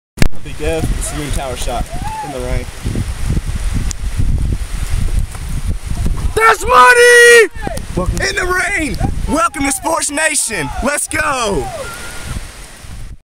Basketball Trickshot off of 120ft Tower (Sports Nation Intro)
Our newest shot off of a 120 foot water tower, we made it in less than 15 tries